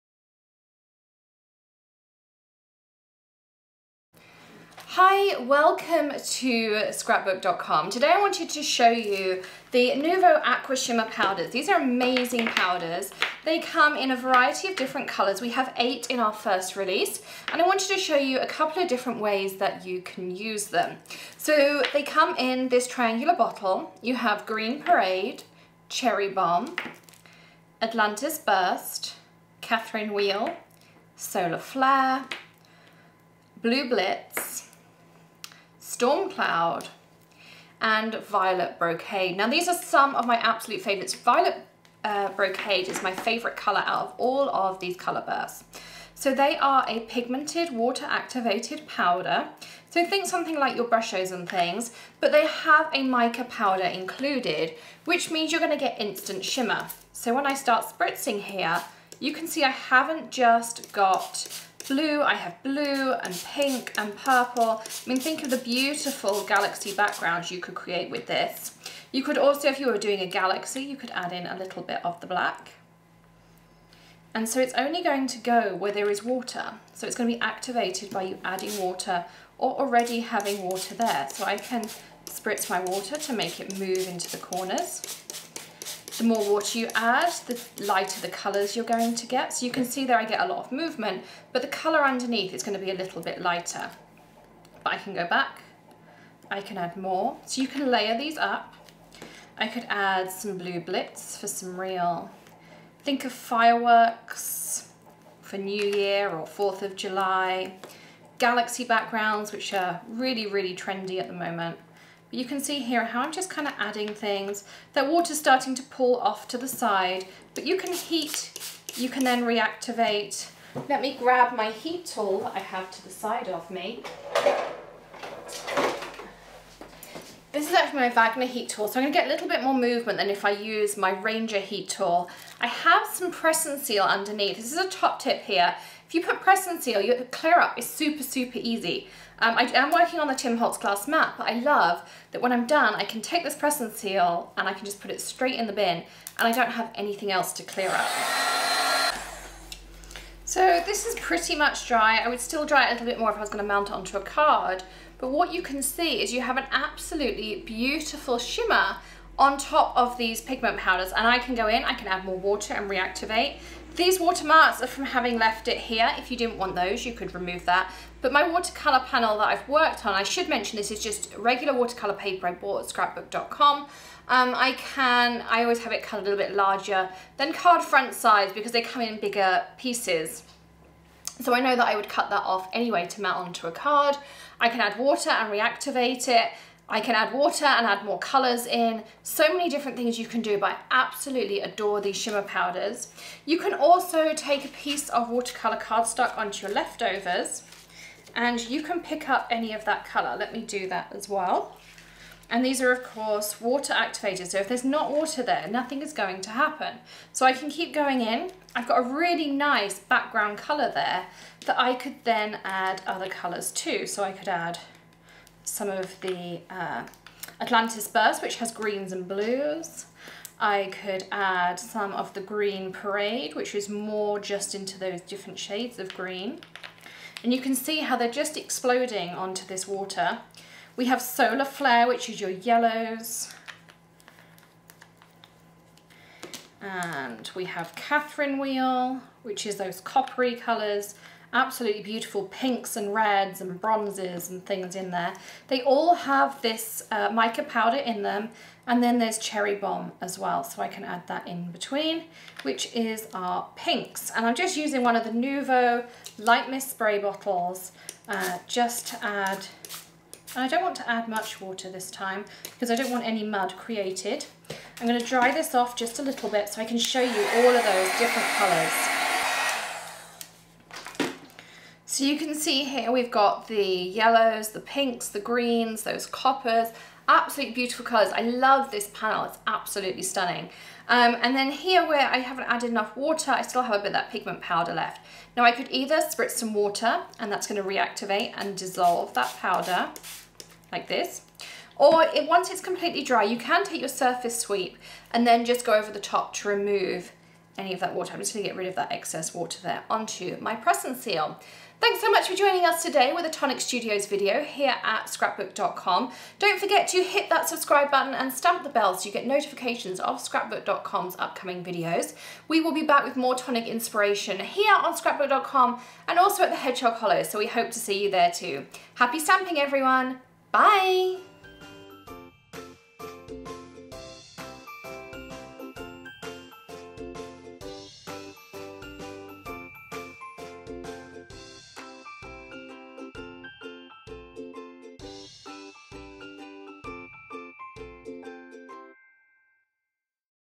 Nuvo Shimmer Powder | Ideas and Inspiration | Tonic Studios
Join Alexandra Stapleton-Smith as she shares tips and techniques for using the Tonic Studios -  Nuvo Aqua Shimmer Powder. Get a burst of color with a touch of shimmer to create stunning backgrounds for cards, layouts, mixed media and more.

• Ranger Ink - Tim Holtz - Distress Watercolor Cardstock - 8.5 x 11 - 10 Pack

• Tonic Studios - Nuvo Collection - Light Mist - Spray Bottle - 2 Pack